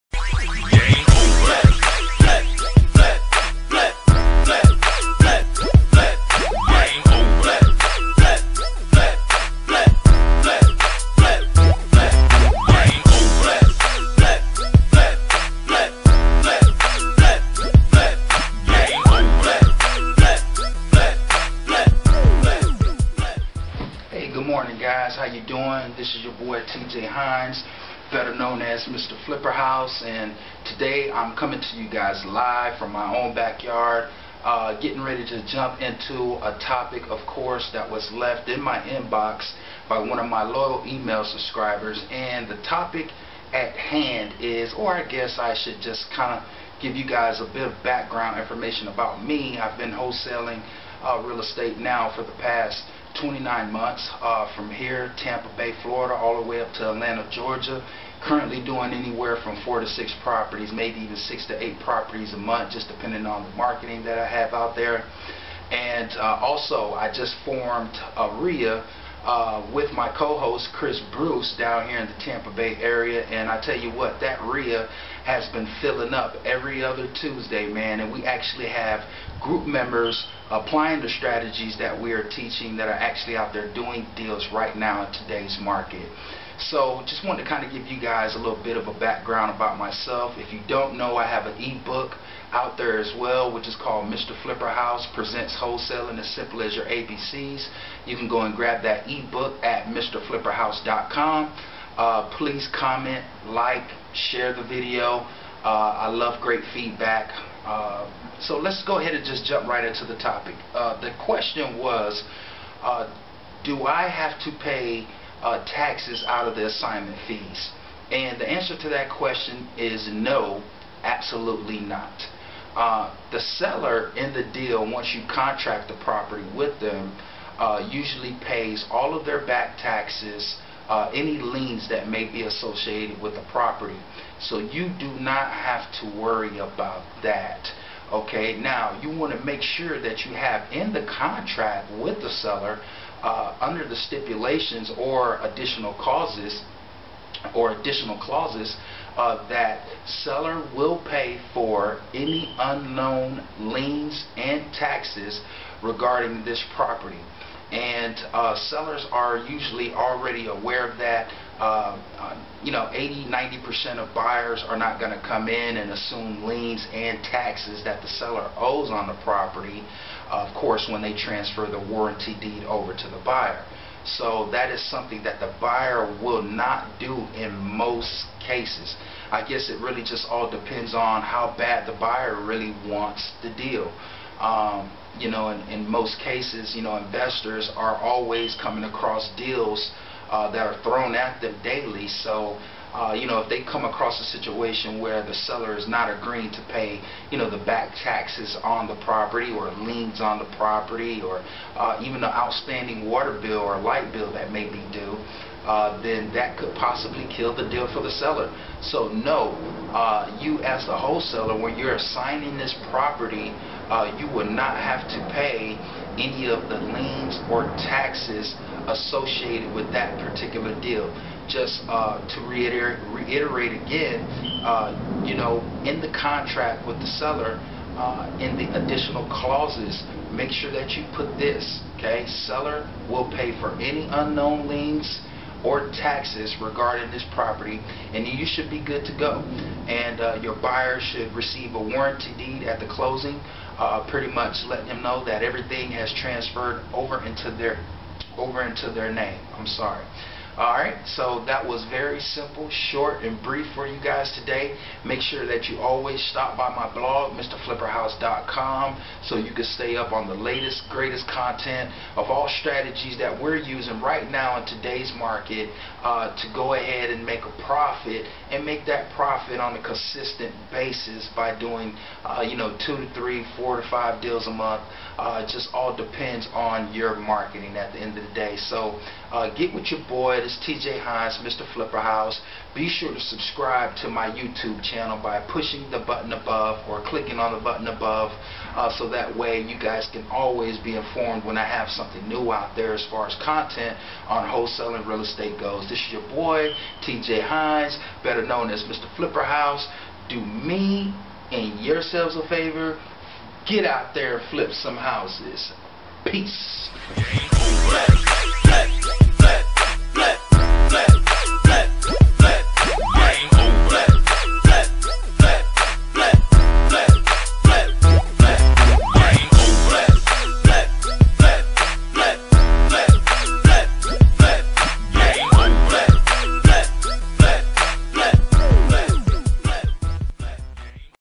Are Taxes Taken Out of The Assignment Fees
Mrflipperhouse explains in this short but sweet video who pays for the taxes in the deal? Is it the Buyer, wholesaler, or seller? Tune in!